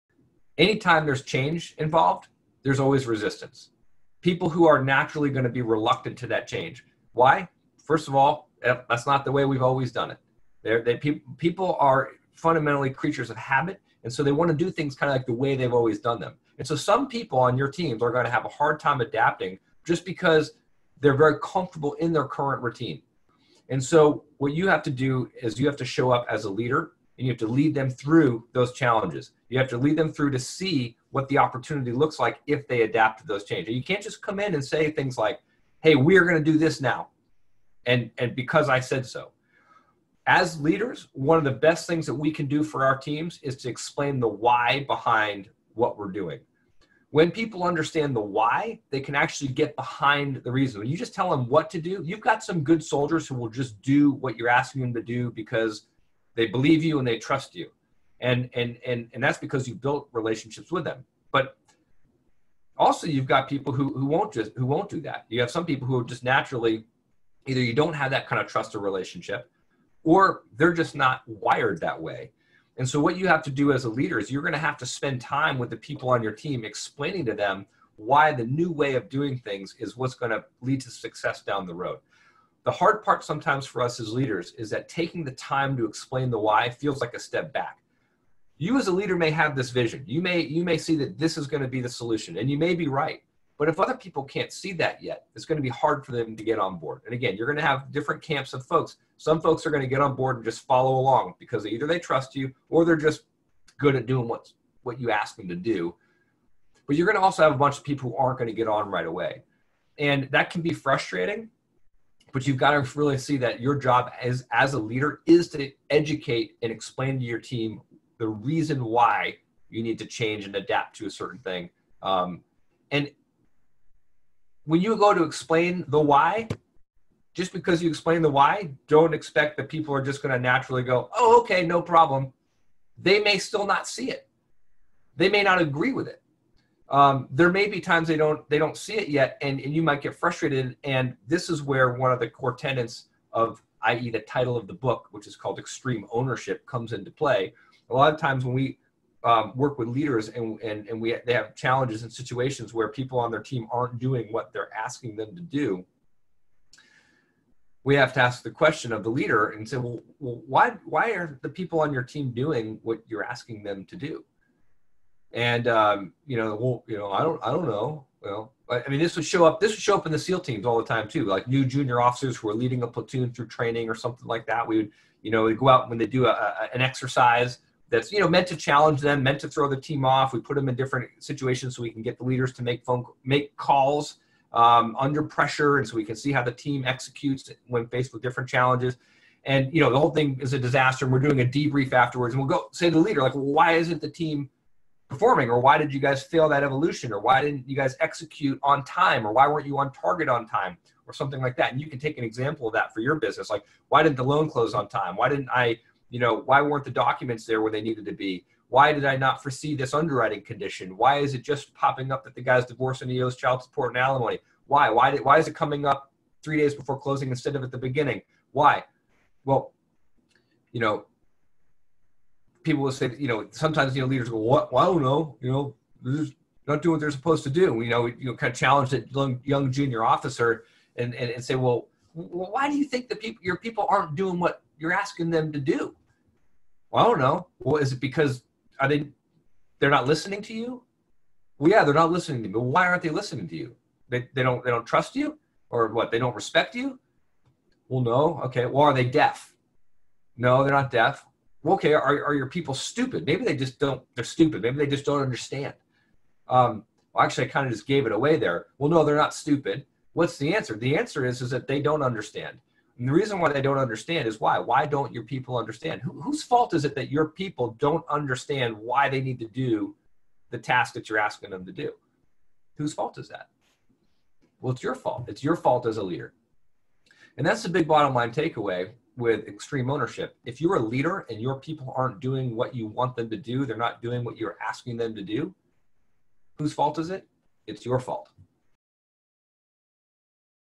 Former Navy SEAL Explains Why Winning and Losing Is Always About Leadership
This former Navy SEAL shares why winning and losing is all about leadership and how you need to take extreme ownership.